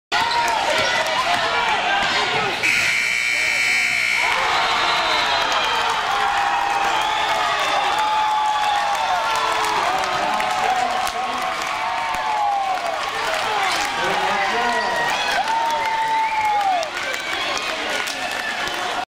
KGhalfcourt.mp4
KG's halfcourt bankshot @ the buzzer